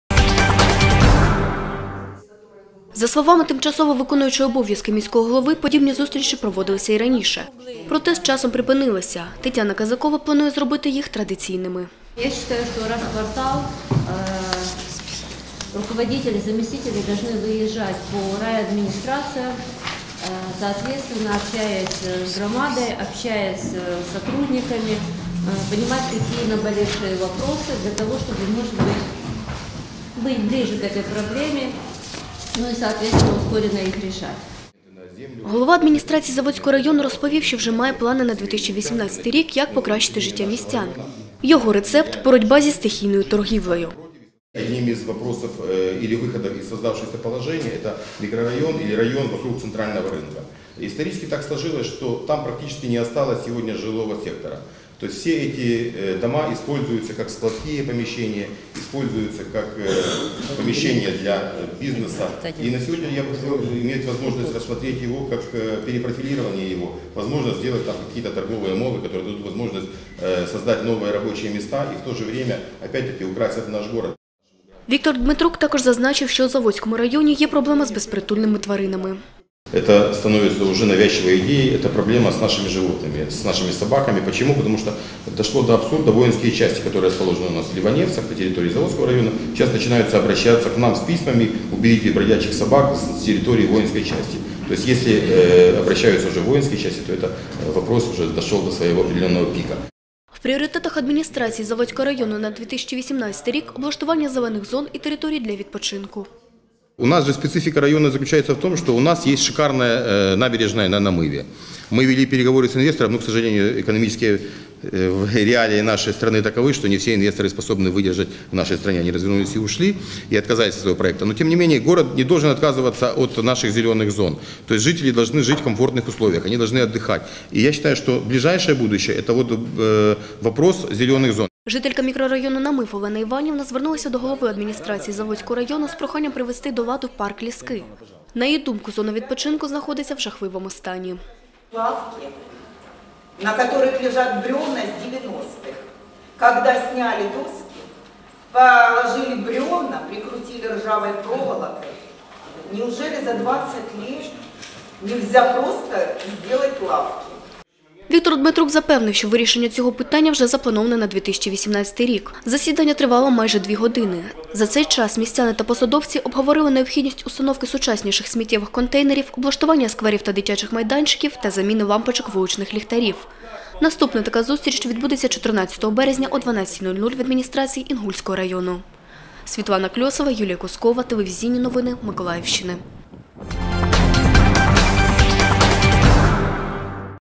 В адміністрації Заводського району відбулась робоча нарада
13 березня в  адміністрації Заводського району відбулась робоча нарада, яку ініціювала виконуюча обов’язки міського голови Тетяна Казакова. Депутати міськради, громадські діячі та жителі району обговорили нагальні питання.